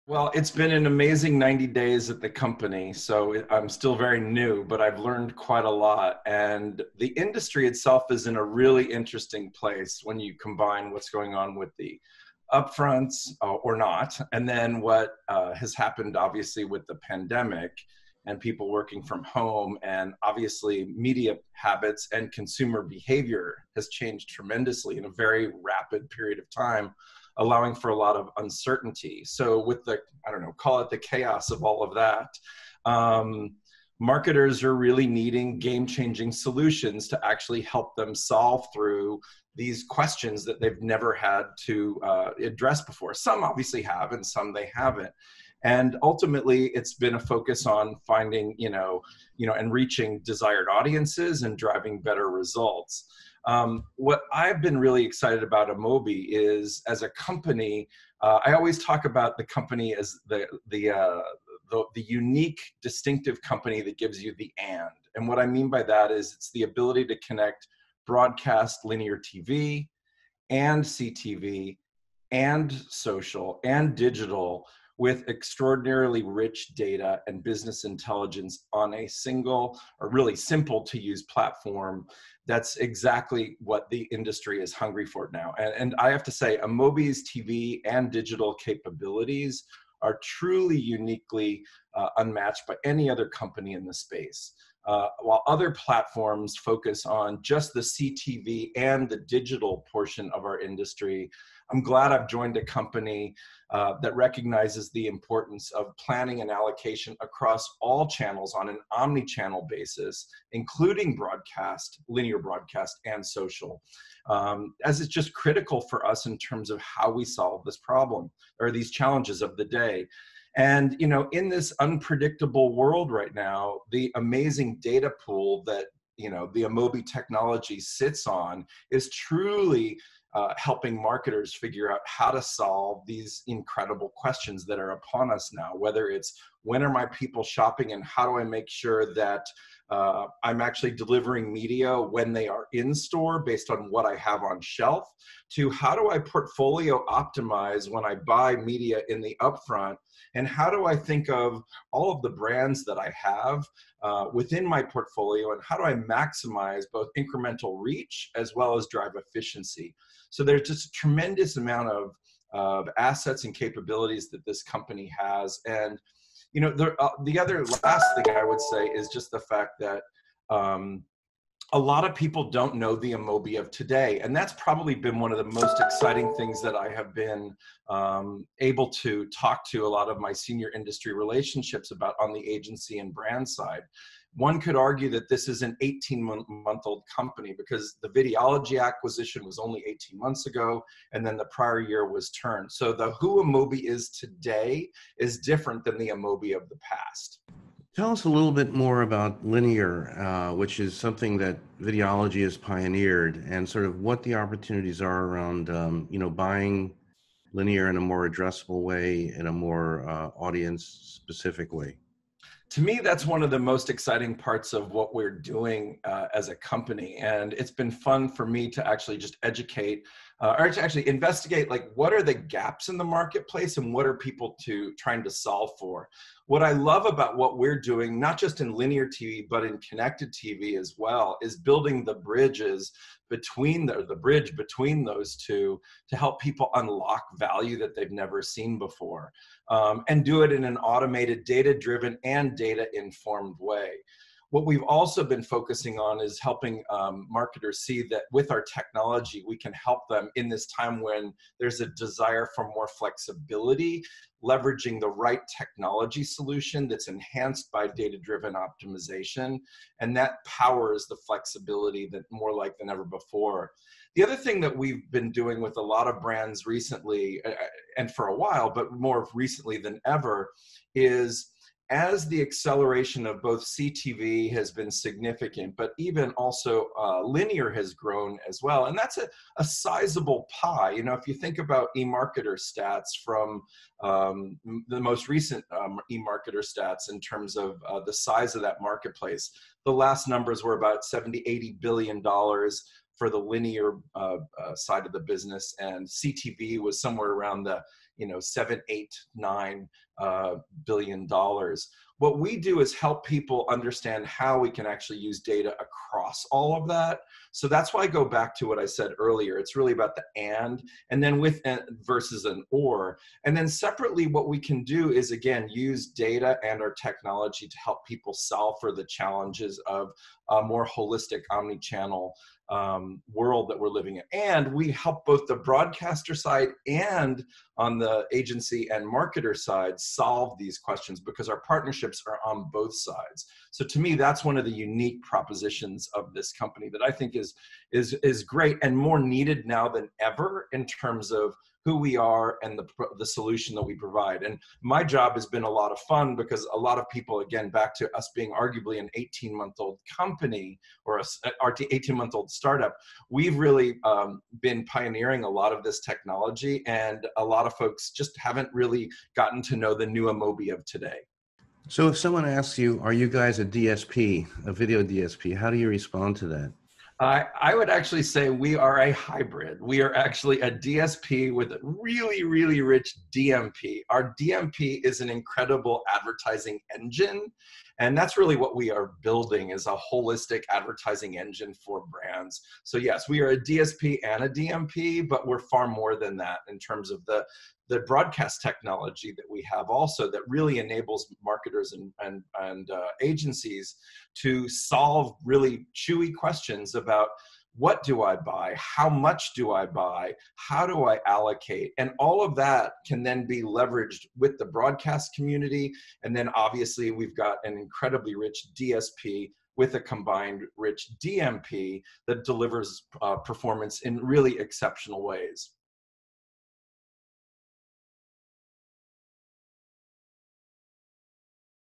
Linear & CTV Go Hand-In-Hand: Amobee's Bamberger
Cord-cutting is still happening at a rate of knots whilst, for many viewers, traditional appointment-to-view TV is a thing of the past.

Amid these shifting audience behaviors, it would be tempting for marketers to contemplate switching from one medium to another.

But emerging evidence suggests a tag-team approach can drive optimal results.

In this video interview with Beet.TV, Jack Bamberger, the chief commercial officer at Amobee, an ad-tech platform whose software helps placement of ads across over-the-top and linear TV, says both are important.

Best of both

"The acceleration of CTV has been significant, but even linear has grown as well," Bamberger says.

"It's a sizeable pie. If you think about the most recent eMarketer stats, in terms of the size of that marketplace, the last (ad spend) numbers were about $70, $80 billion for the linear side of the (TV ad) business and CTV was somewhere around $7, $8, $9 billion.

"What we do is help people understand how we can actually use data across all of that. It's really about the 'and' versus an 'or'.

"The ability to connect broadcast linear TV  and CTV and social and digital with extraordinarily rich data and business intelligence... that's exactly what the industry is hungry for now."

New commercial chief

Amobee is a media management software provider helping in the use of data for planning, transacting, measuring and analyzing ads across TV, digital and social.

Amobee, whose TV initiatives took off thanks to its earlier acquisition of Videology Group, helps advertisers buy and sell across 30-second connected TV and other video inventory.

Bamberger joined Amobee in April after serving as Verizon Media's VP of global partnerships, bringing relationships with global agencies and other accounts following earlier stints at MEC, Dentsu, Meredith, Time Warner and IPG.

The road ahead

EMarketer now expects US TV ad spending revenue will have taken a dive during the first half of the year, thanks partly to COVID-19, leaving a bigger shortfall to make up through the year if it is not to fall below its typical $70 billion threshold.

All of which means the emergence of connected TV ad opportunities and even data-driven linear may have an important role to play - bringing higher effectiveness that could turn TV into a more performance-driven medium, encouraging agencies to return to spending.

Bamberger says advertisers want to know where viewers are shopping, how to deliver ads to them there based on in-store inventory and how to optimize upfront-bought media.

"In this time, there's a desire for more flexibility, leveraging the right technology solution that's enhanced by data-driven optimization," he says.